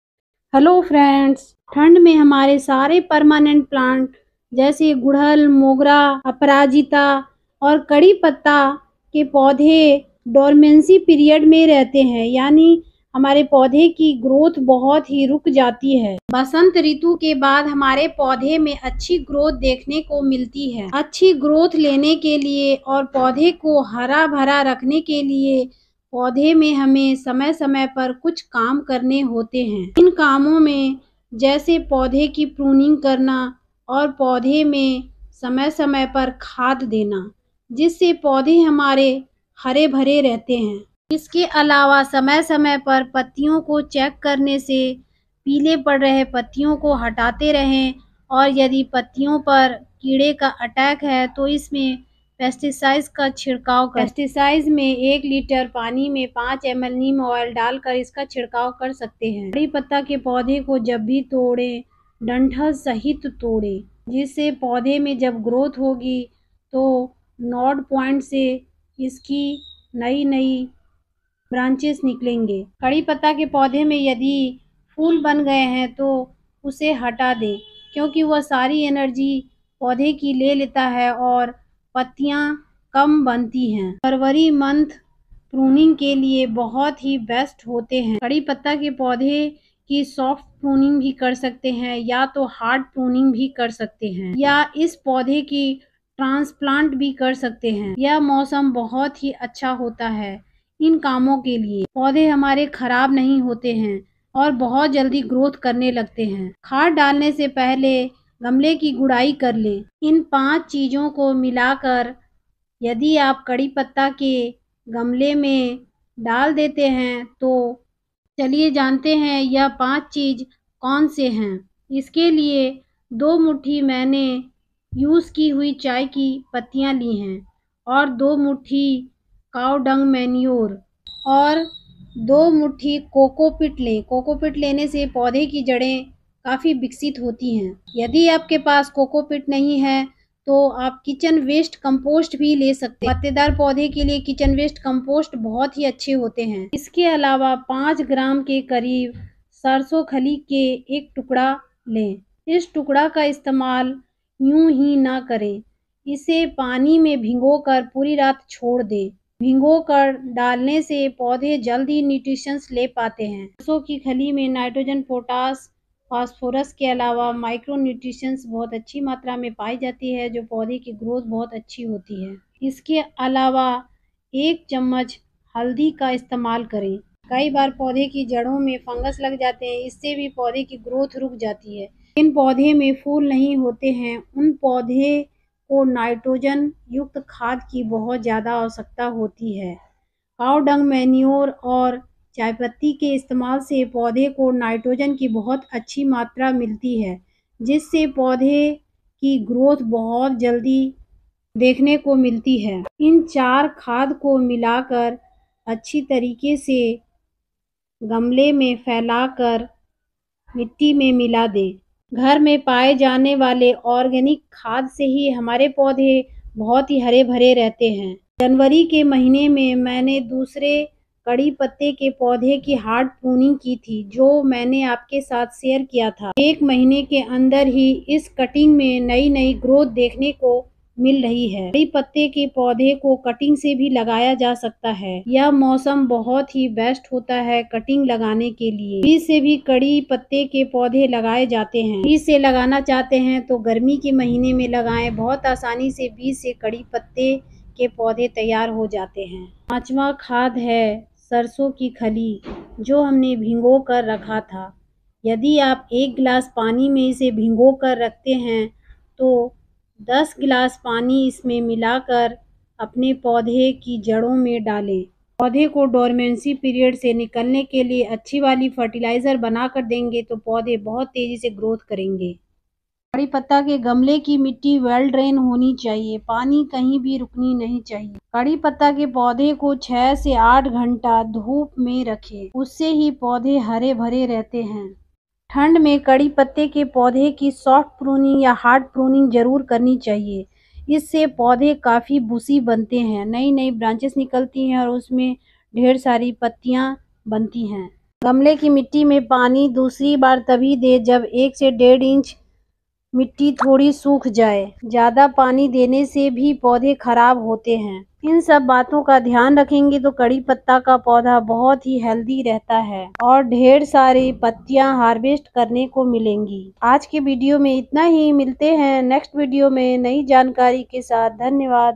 कड़ी पत्ता के पौधे कि जड़ में पाँच चीज मिला कर डाले पड़ोसी पूछेंगे हरा और घना होने का राज#mitha nim
kadi patta ke paudhe me panch chij mila kar dale parosi bhi puchenge ghana aur hara hone ka raj
Mitha nim ke paudhe ki care kaise kare
How to care curry leaf plant
curry patta/mitha nim/care/cutting plant/fertiliser/ medicine plant /lucky plant for home garden
gardening for mental peace 🙏